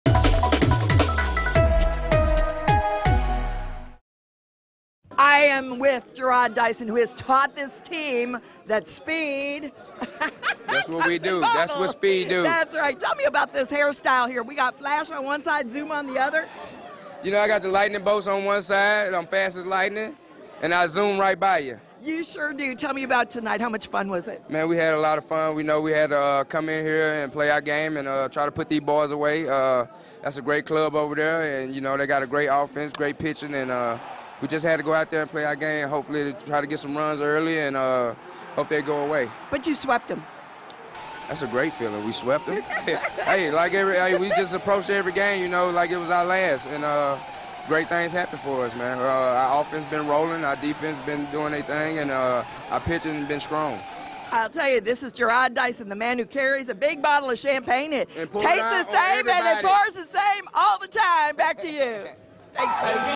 Watch: Royal Jarrod Dyson dumps champagne on Karen Kornacki's head
KMBC's Karen Kornacki joined the Kansas City Royals' celebration last night when Jarrod Dyson dumped champagne on her head.